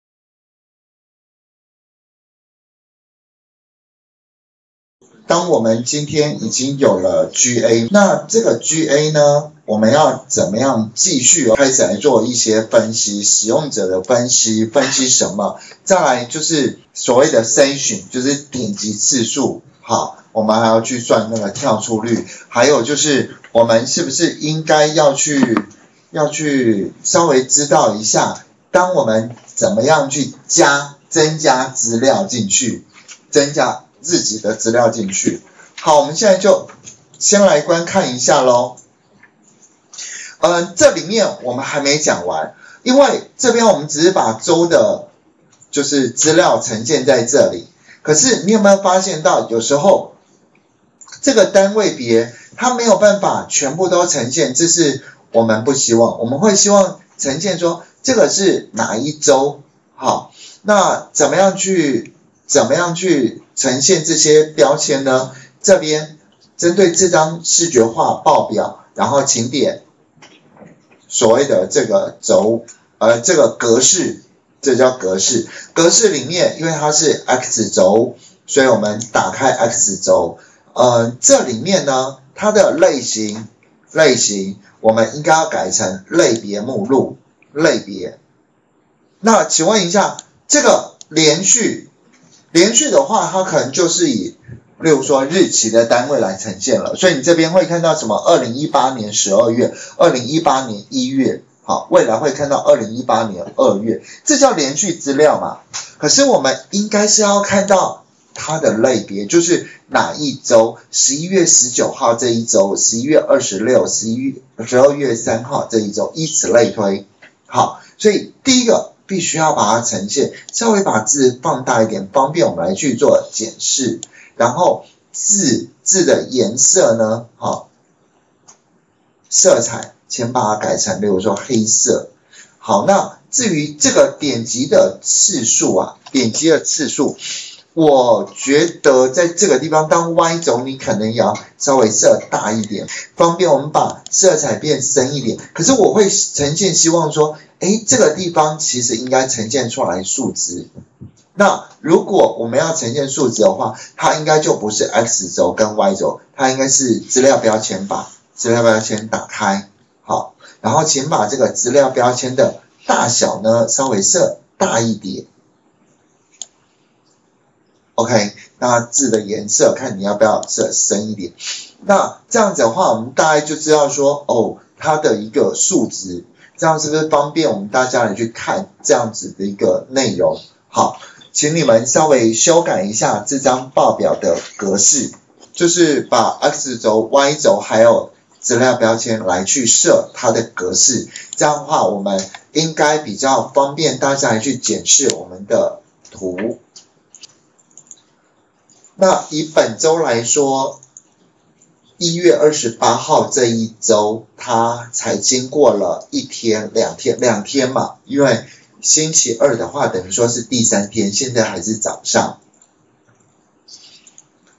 55.報表的格式設定-類別X軸、數值Y軸、資料標籤(用大數據分析做GA網站分析)
55.報表的格式設定-類別X軸、數值Y軸、資料標籤(用大數據分析做GA網站分析)
Google Analytics 是一個Google開發的免費線上工具，主要功能是進行網站資料分析，並以視覺化分析呈現結果。會使用GA工具並不代表會網站分析，因為每一個人都可以輕易申請好GA的使用權，都可以輕易看到GA的報表，視覺化分析是很重要的學習重點。
如何進行網站分析、分析什麼?這才是本門課的重點。本課程以自己的網站被瀏覽次數為資料來源，使用Power BI軟體做資料分析，並找出行為模式，找出瀏覽者地理位置、瀏覽行為、喜歡哪些網頁；真正達到自己的網站、自已分析的目的。